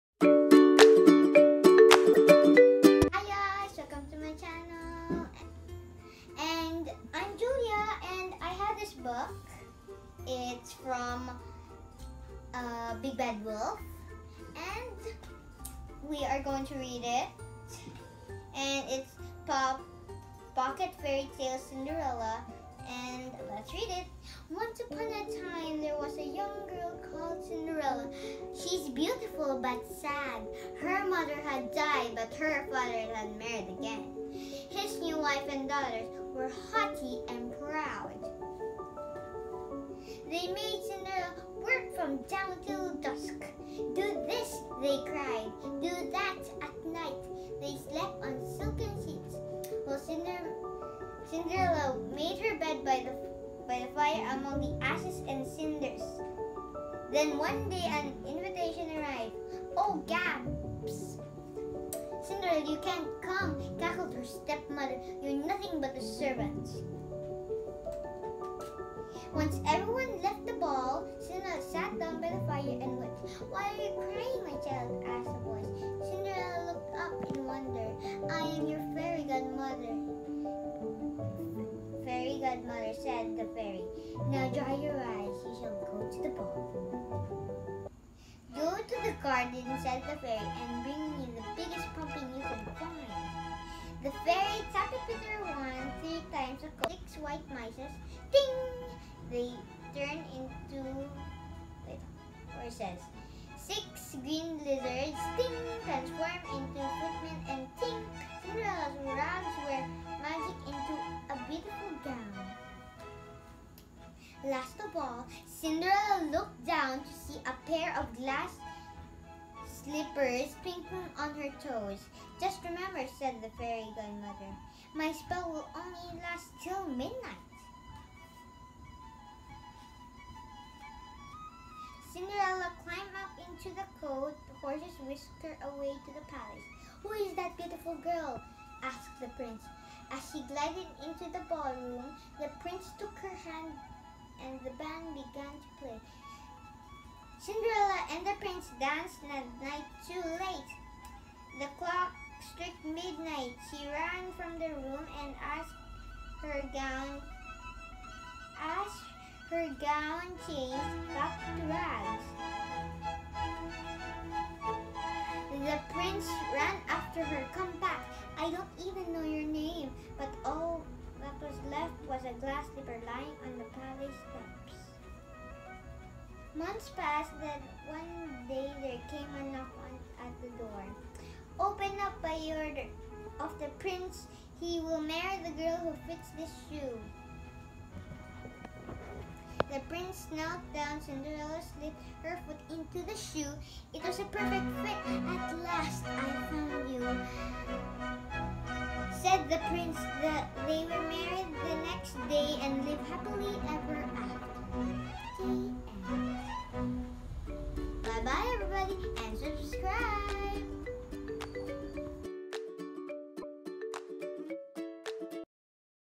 Cinderella Story
Bought a Cinderella Story Pocket Book @ BigBadWolf 🤩